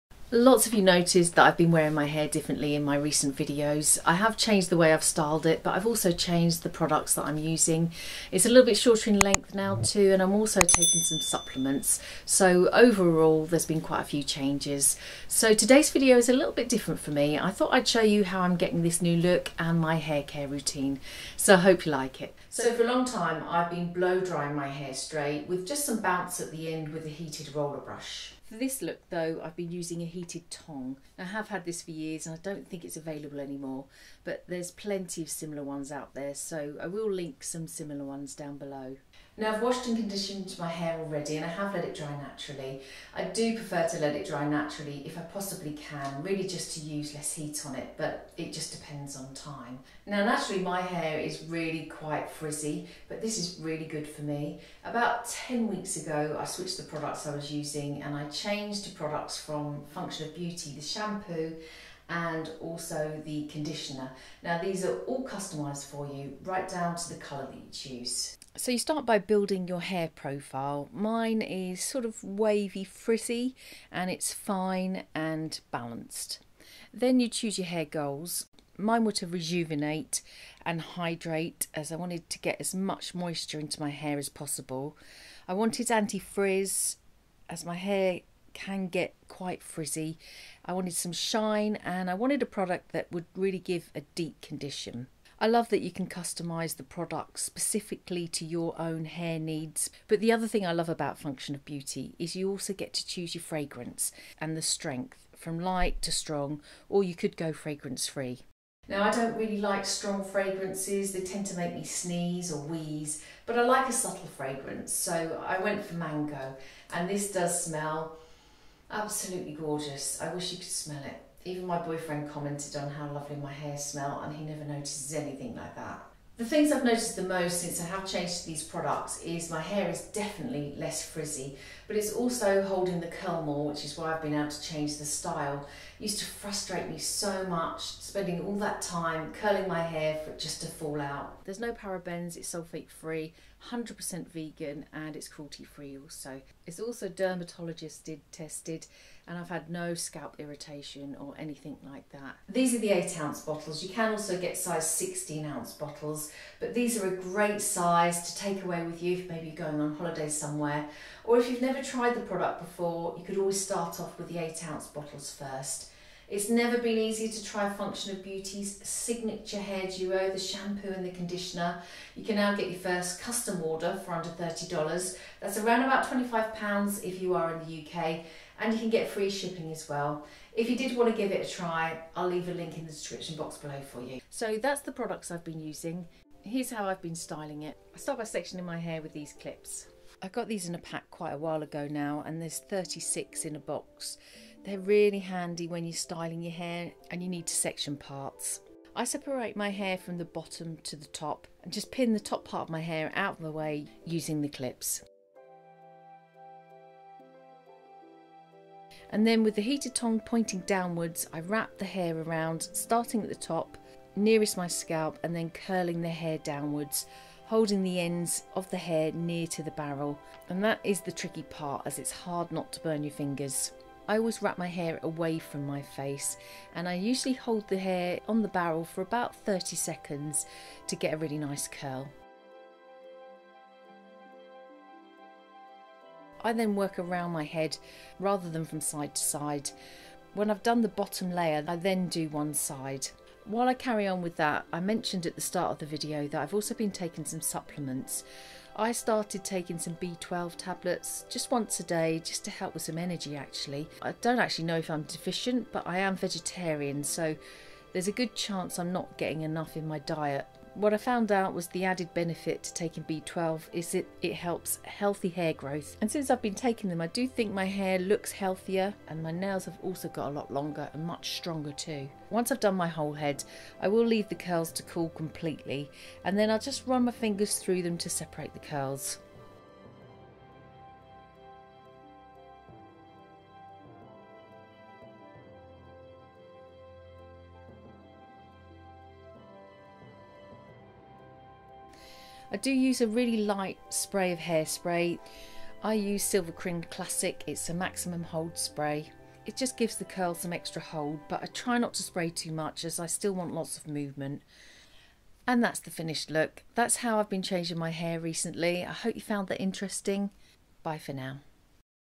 My Journey to Less Frizzy More Healthy Hair at 50 plus Soft Waves Hair Tutorial
I've changed my hairstyle! Watch as I show you my new hair care routine; the products I'm using & loving, the supplements I've been taking to make my hair look and feel much healthier and shinier and a full soft waves hair tutorial .

Hair Tongs (similar)
Hair Tongs (similar)
B12 (similar)

❤ “WORKOUTS FOR WOMEN OVER 40”